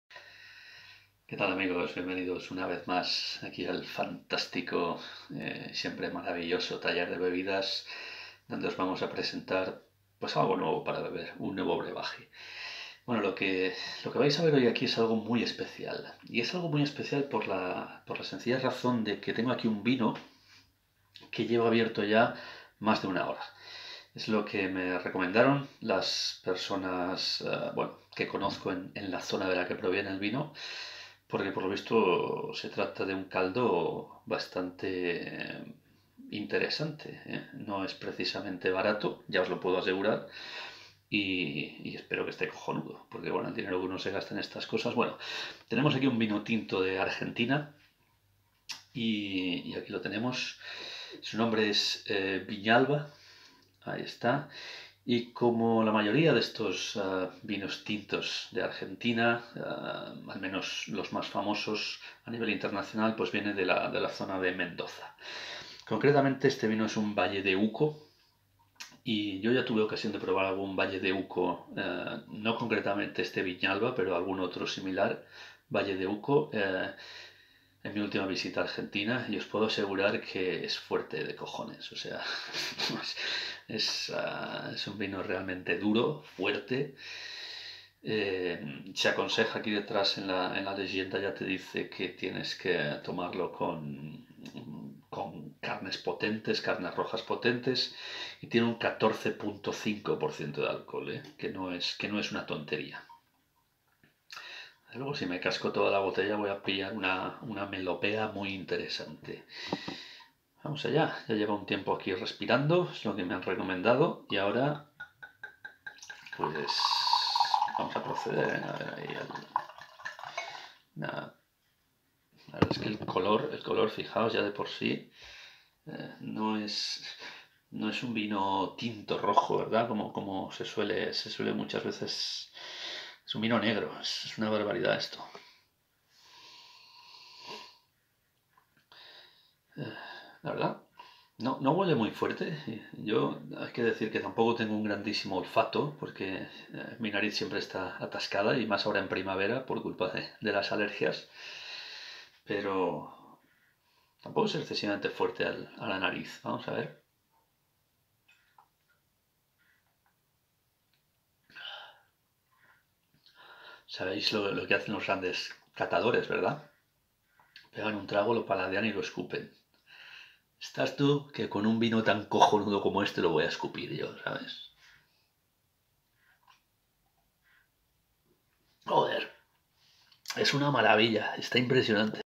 Viñalba. Reserva 2018 🇦🇷
Vino tinto de Mendoza, Argentina. Mezcla de uvas Malbec y Touriga Nacional. 14.5% de alcohol.